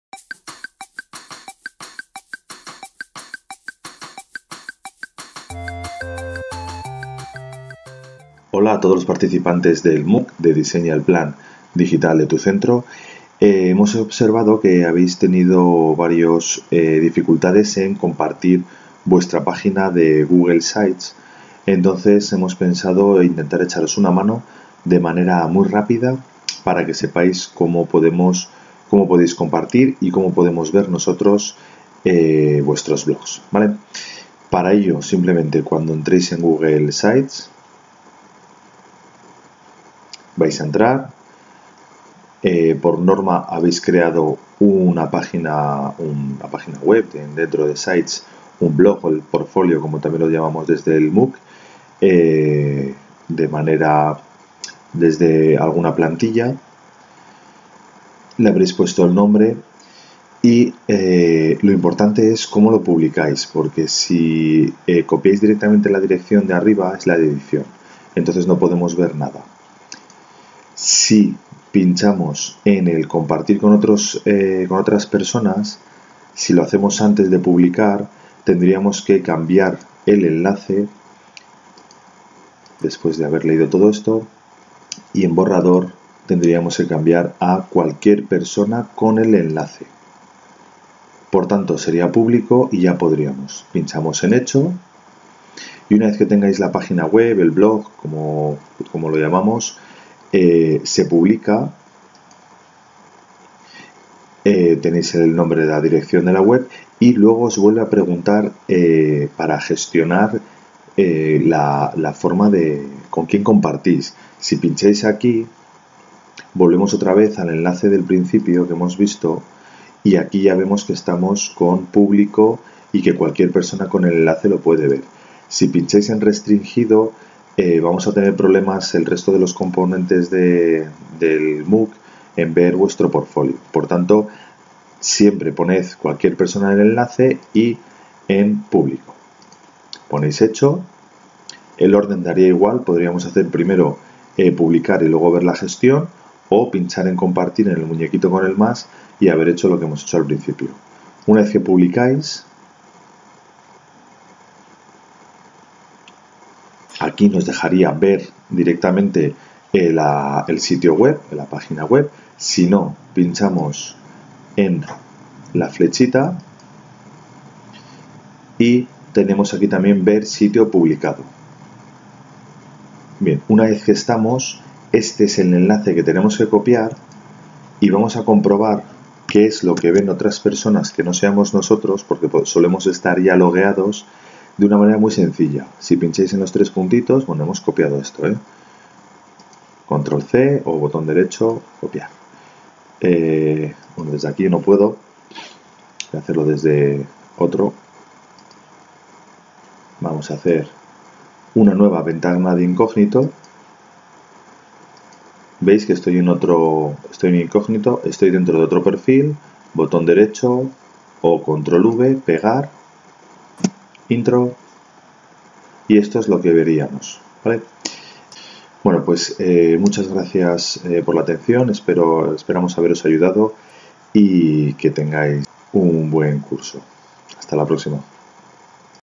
Publicar un blog en Sites Google y que lo vea cualquier usuario
Pequeño "tip" para saber cómo publicar una web de Sites de Google con permisos abiertos y para que cualquier persona pueda verlo.